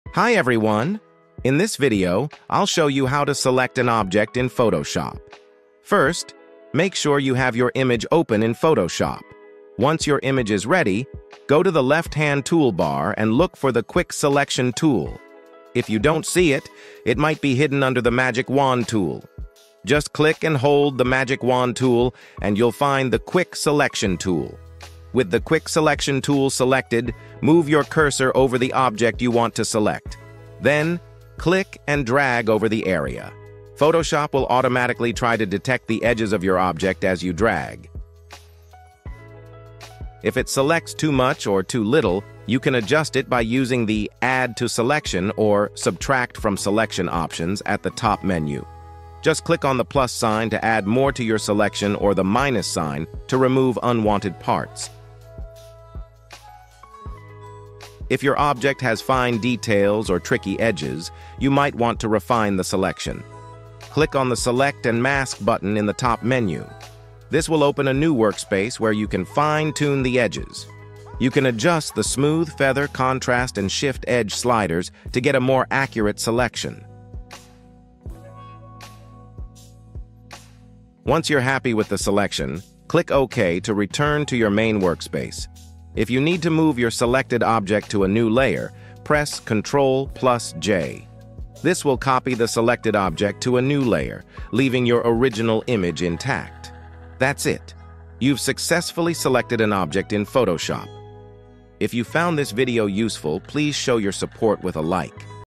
How to Select an Object in Photoshop (Easy Tutorial)
Welcome to our detailed guide on How to Select an Object in Photoshop! This video is designed to help both beginners and seasoned users master the techniques needed to accurately select objects within your projects using Adobe Photoshop.

Timestamps:

00:00 - Opening your image & selecting the quick selection tool
00:13 - How to find the quick selection tool
00:28 - Using the tool to select your object
00:41 - Adjusting your selection
00:57 - Refining the selection with select and mask
01:12 - Finalizing your selection
01:29 - Moving the selected object to a new layer
01:43 - Conclusion & how to support the channel

What You Will Learn:

How to open and prepare your image for editing.

The steps to locate and use the quick selection tool effectively.

Techniques for adjusting and refining your selection to include precise details.

How to use the select and mask workspace to fine-tune the edges of your selection.

Tips for moving your selected object to a new layer, keeping your original image intact.

This tutorial ensures you will have a firm grasp on how to select an object in Photoshop by the end, streamlining your editing process and enhancing your overall Photoshop skills.